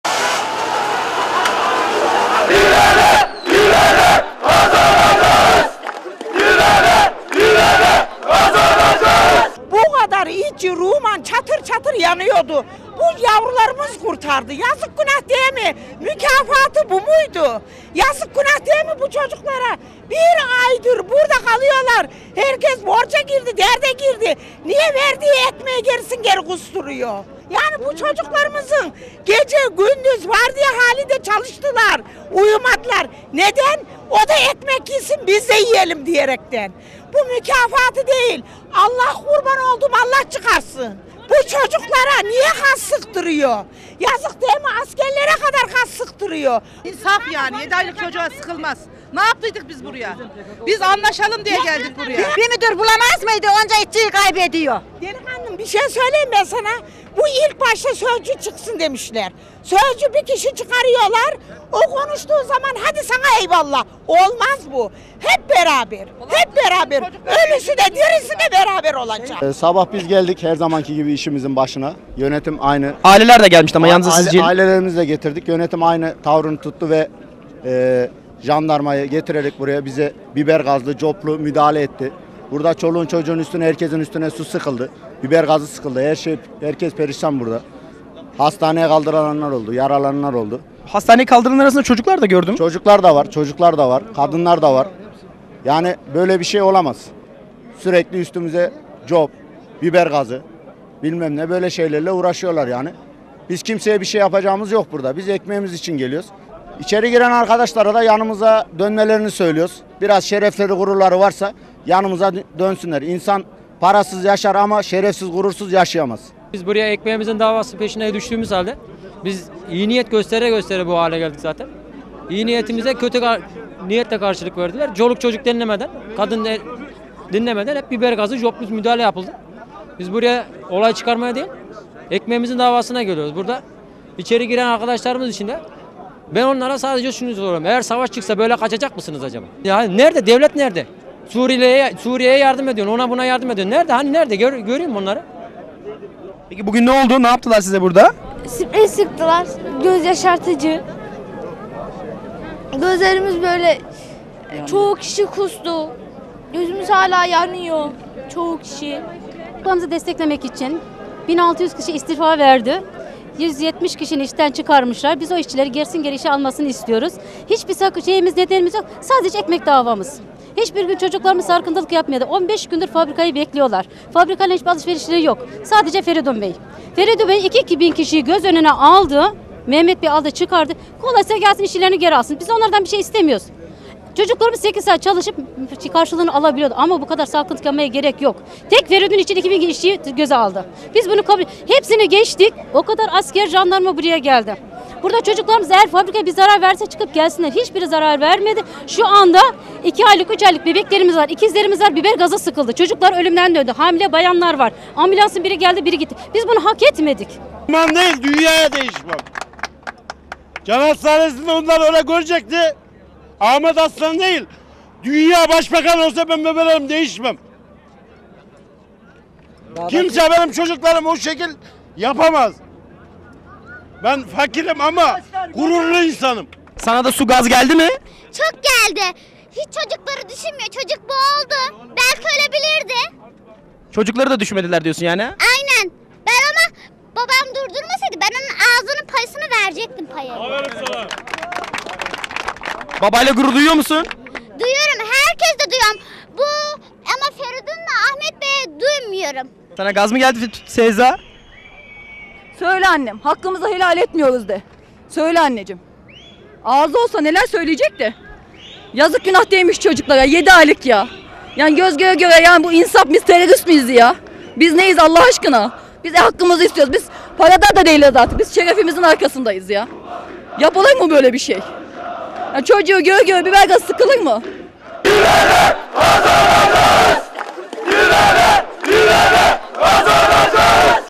ORS işçilerine jandarma saldırısı
Ankara Polatlı’da ORS işçilerinin mücadelesi devam ediyor. Patron direnişi kırmak servislerle fabrikaya işçi sokmak istedi. Ancak direnişteki işçiler patrona engel olmaya çalıştı. Direnişteki işçilere jandarma biber gazı, cop ve tazyikli suyla saldırdı. İşçiler ve ailelerinden yaralananlar, fenalaşanlar oldu. Saldırıyı ve tepkilerini işçiler ve işçi aileleri televizyonumuza anlattı. İşçiler direnişte kararlı!